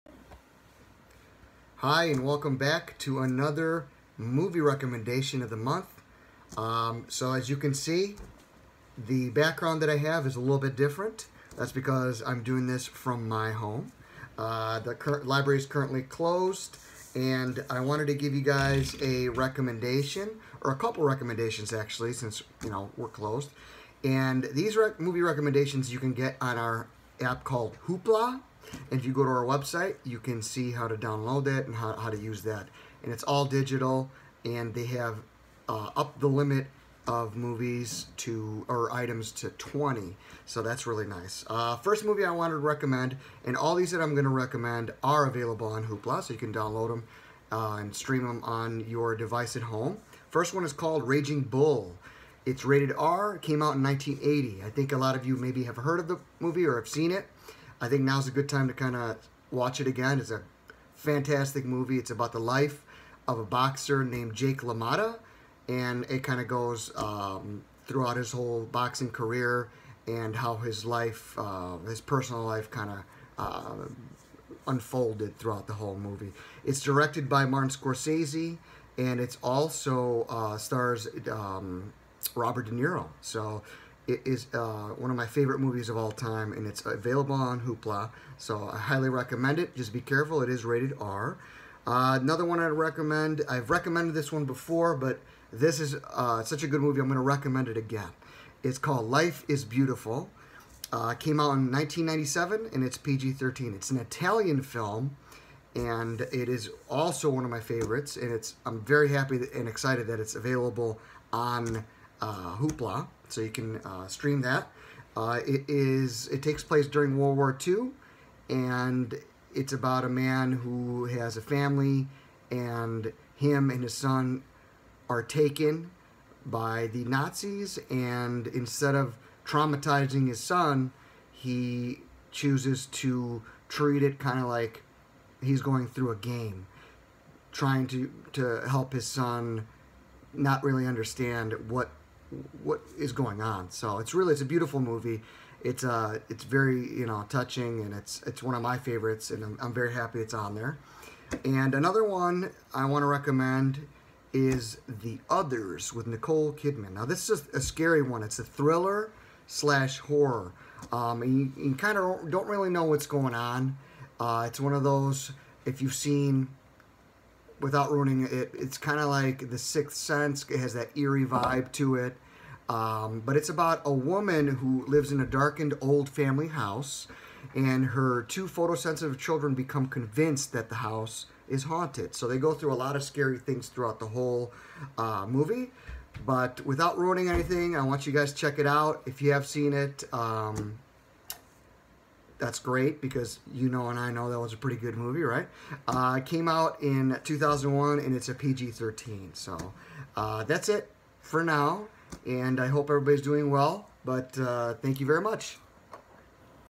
Movie of the Month: April 2020
Our Movie's of the Month include: Raging Bull, Life is Beautiful and The Others. All are available on Hoopla